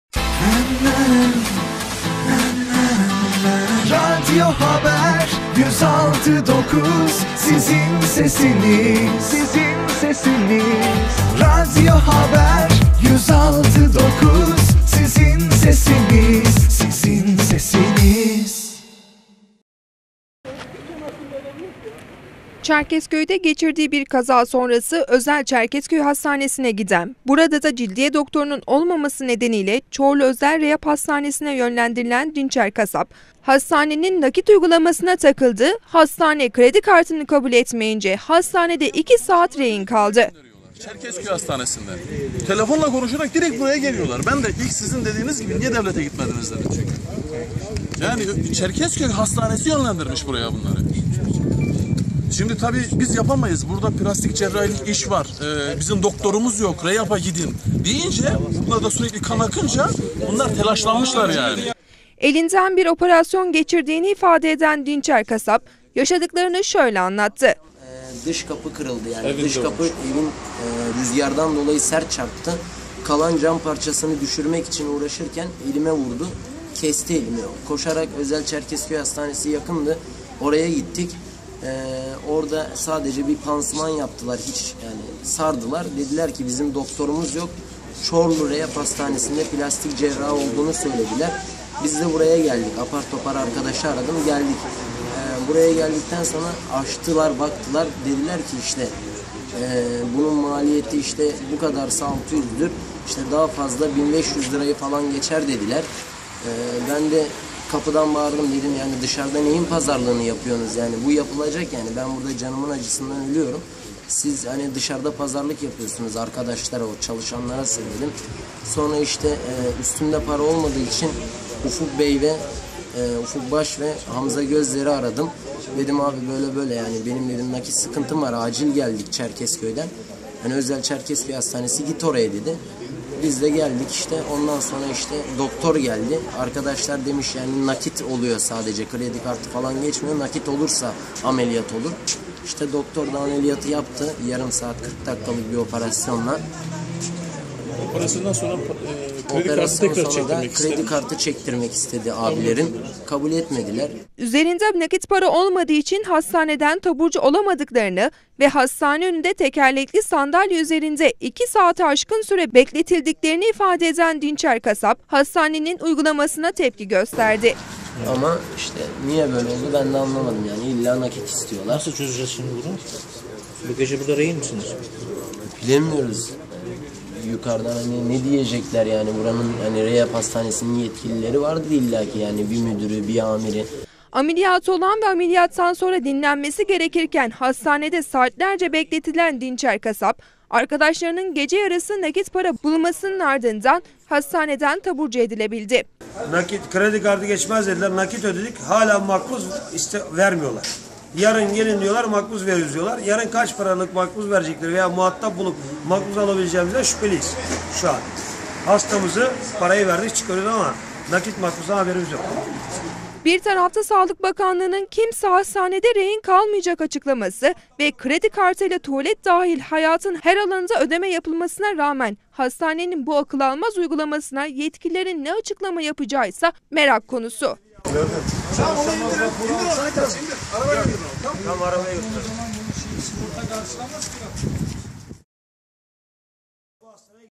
HASTANE KREDİ KARTINI KABUL ETMEYİNCE, REHİN KALDI
ÇERKEZKÖY'DE GEÇİRDİĞİ BİR KAZA SONRASI ÖZEL ÇERKEZKÖY HASTANESİ'NE GİDEN, BURADA DA CİLDİYE DOKTORUNUN OLMAMASI NEDENİYLE ÇORLU ÖZEL REYAP HASTANESİ'NE YÖNLENDİRİLEN DİNÇER KASAP, HASTANENİN 'NAKİT UYGULAMASI' UYGULAMASINA TAKILDI...HASTANE KREDİ KARTINI KABUL ETMEYİNCE, HASTANEDE REHİN KALDI...